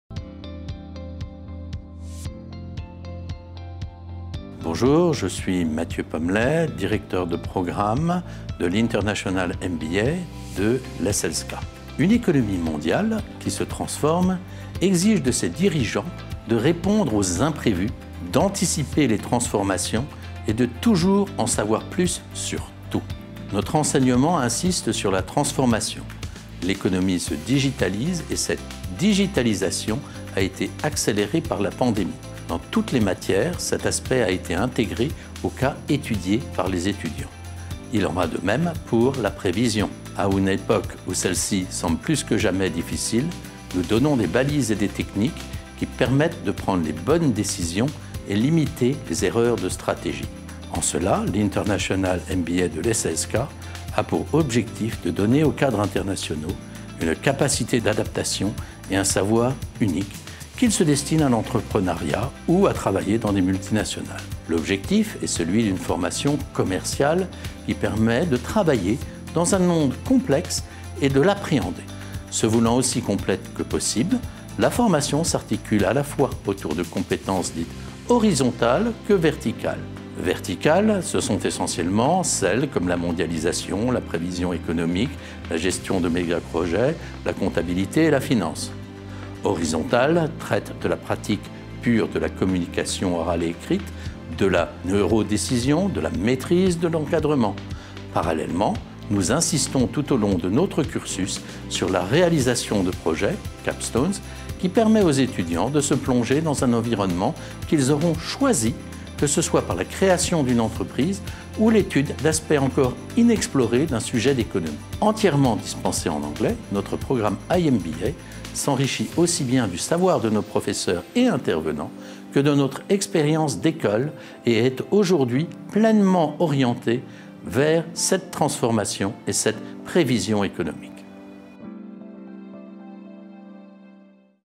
Présentation de l'International MBA par Mathieu Pommellet
Découvrez l'International MBA à l'ESLSCA Business School Paris présenté par Mathieu Pommellet, le directeur de programme.

Discover the International MBA at ESLSCA Business School Paris presented by Mathieu Pommellet, the program director.